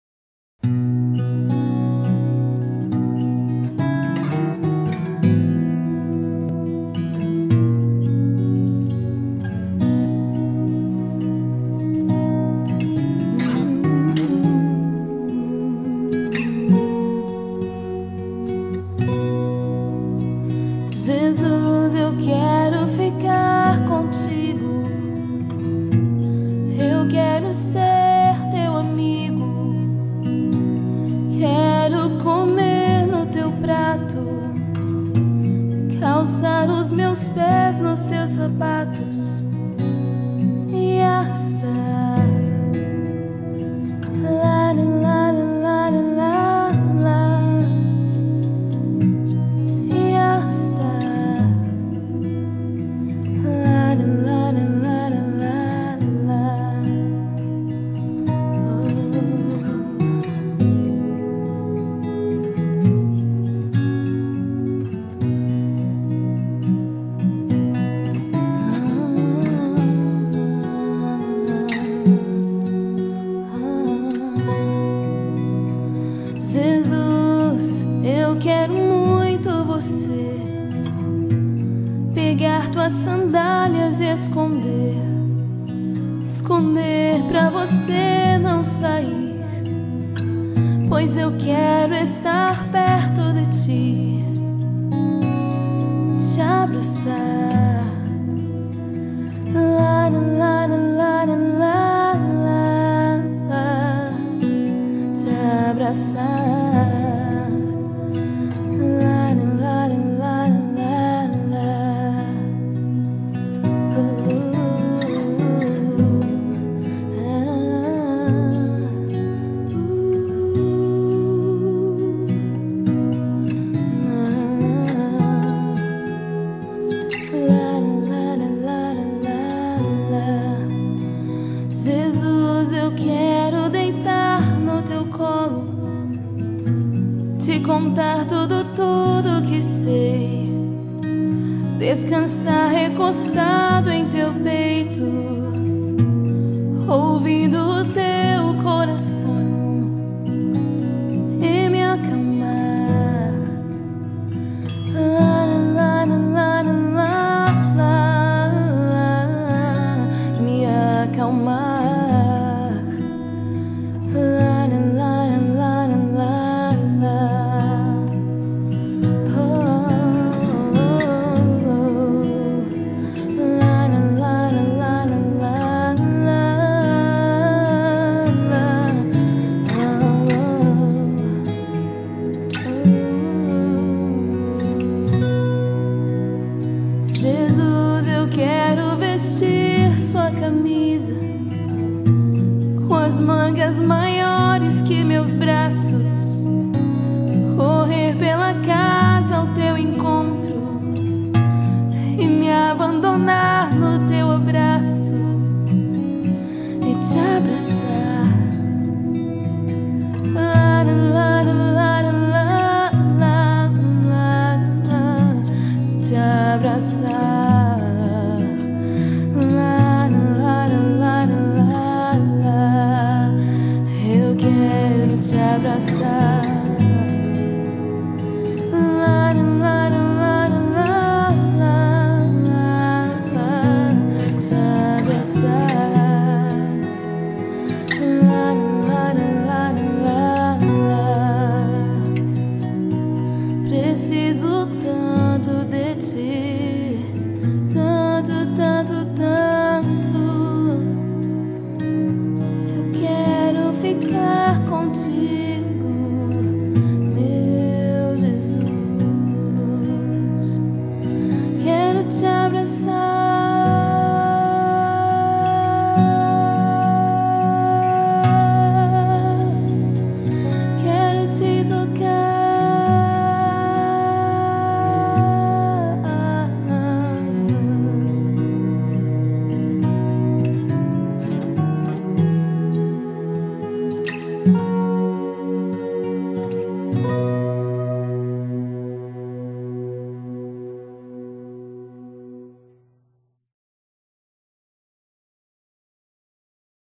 Aniversário da Thays
Vídeo exibido no Aniversário de 15 anos da Thays da Rosa Costa realizado dia 12/10/09 na Churrascaria Freio de Prata em Canoas. Comemorado por amigos parentes e a igreja em uma grande festa.